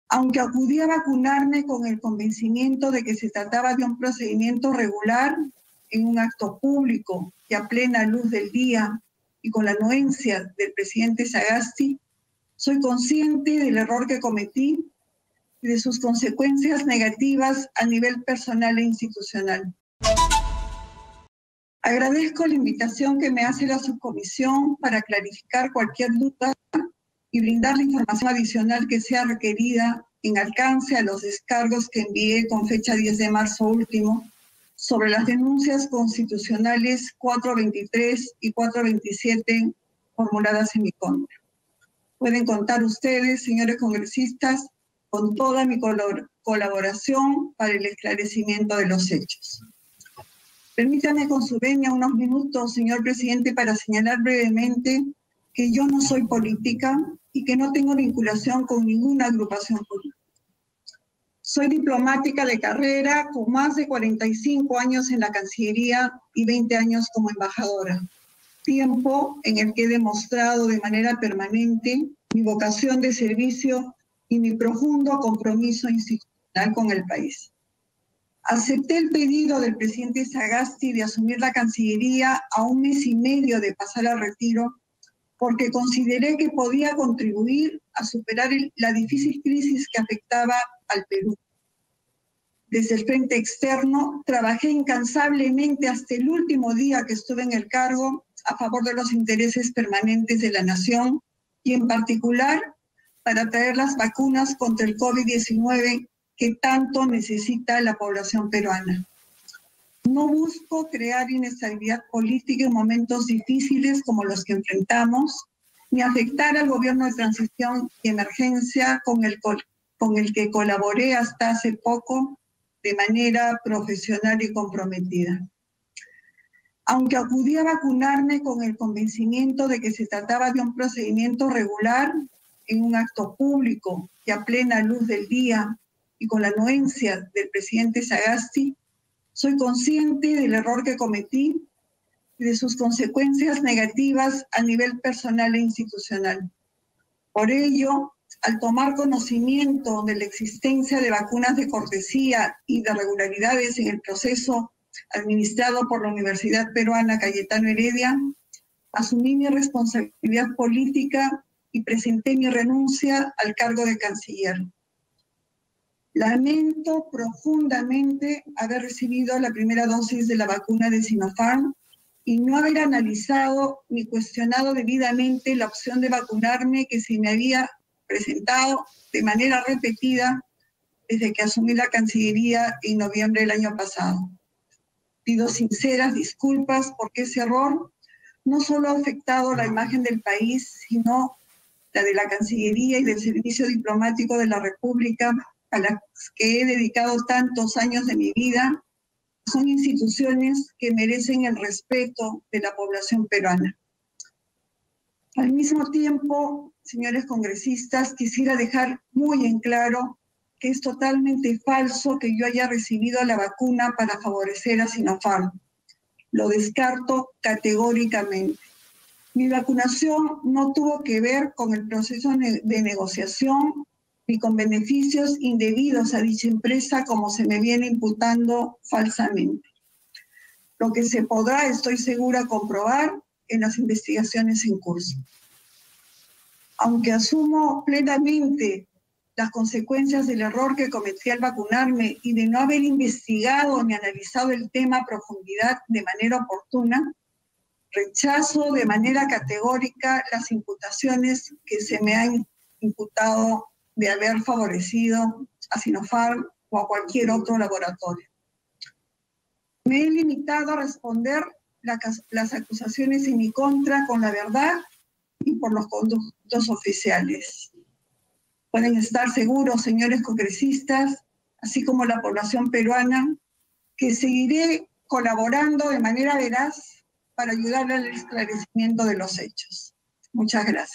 🔴🔵Exministra Astete ratificó que presidente Francisco Sagasti aprobó su vacunación
Durante su participación en la Subcomisión de Acusaciones Constitucionales del Congreso, que investiga el caso Vacunagate, la exministra de Relaciones Exteriores, Elizabeth Astete, ratificó esta mañana que el presidente Francisco Sagasti aprobó su vacunación.

Exitosa Perú
"La voz de los que no tienen voz"

Corporación Universal